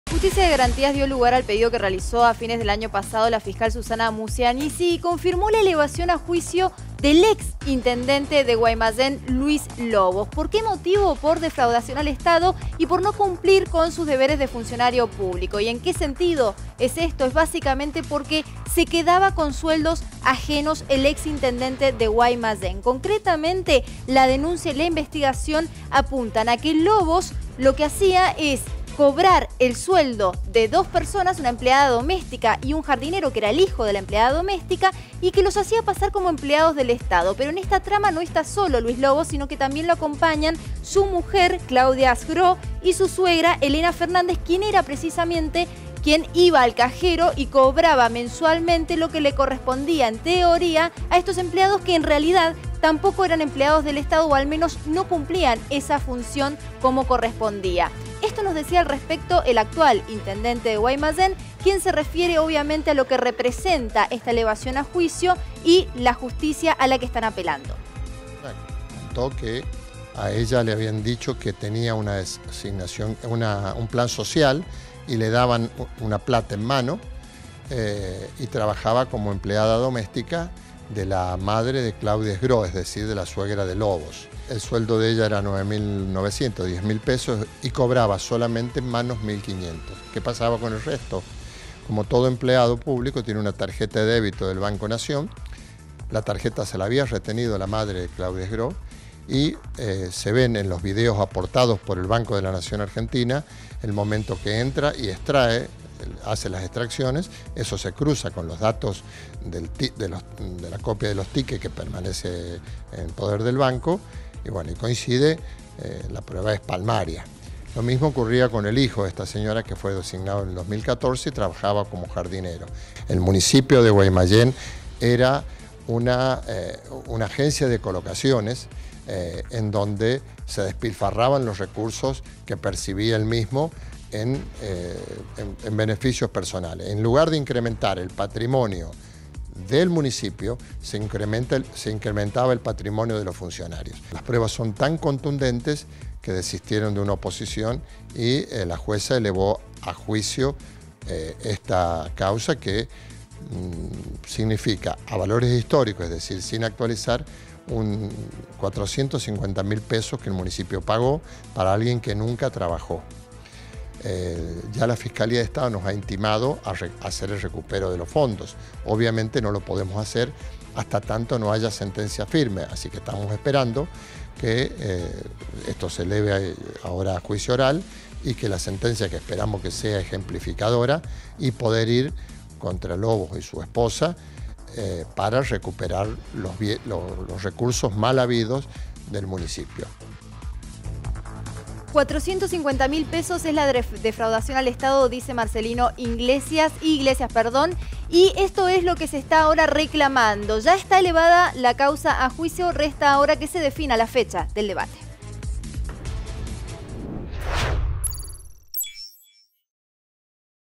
Lobos va a juicio por quedarse con el sueldo de su empleada
El ex intendente de Guaymallén está acusado de contratar empleados fantasmas y no pagarles el sueldo.